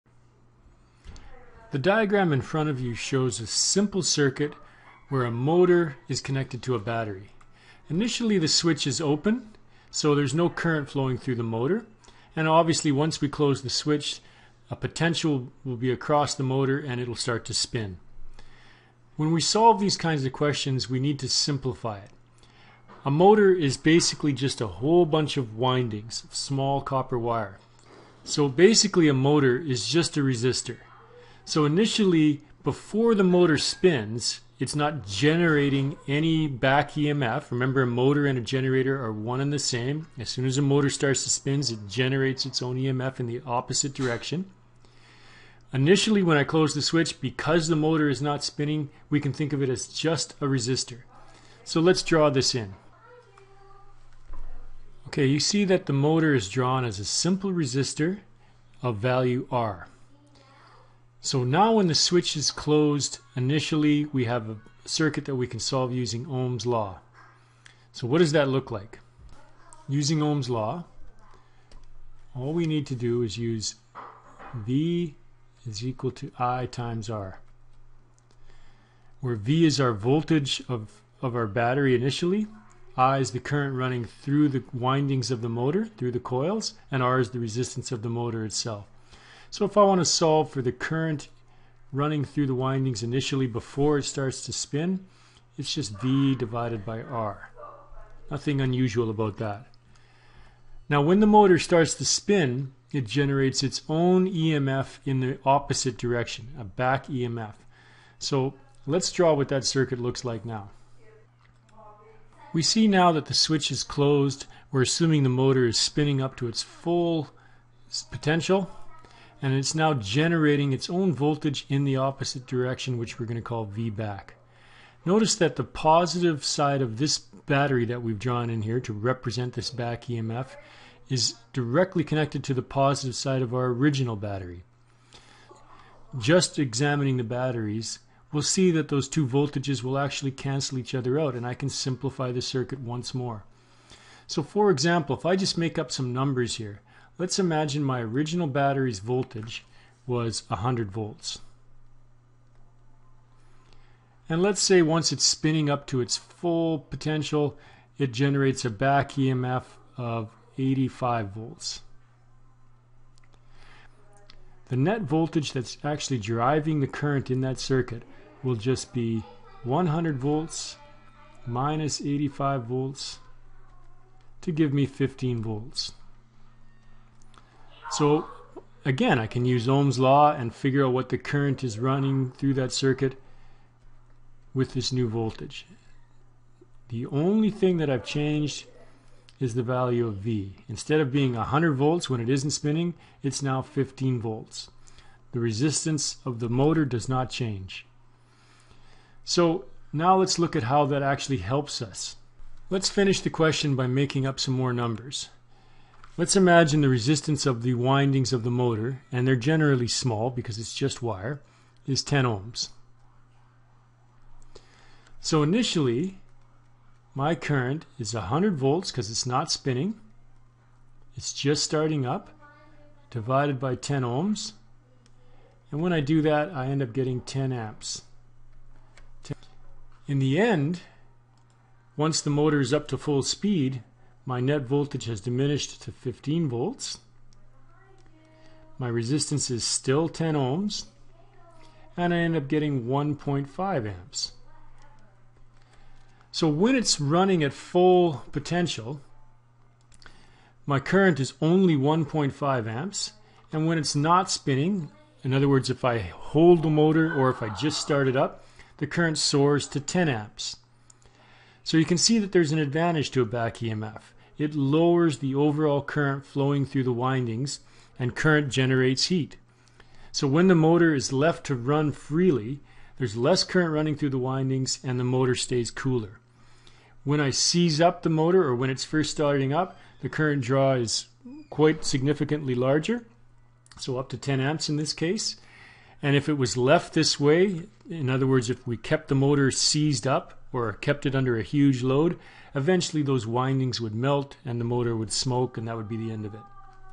Back Emf of a DC Motor
When a motor spins it generates a back emf.  Let's see how this affects the current through the windings.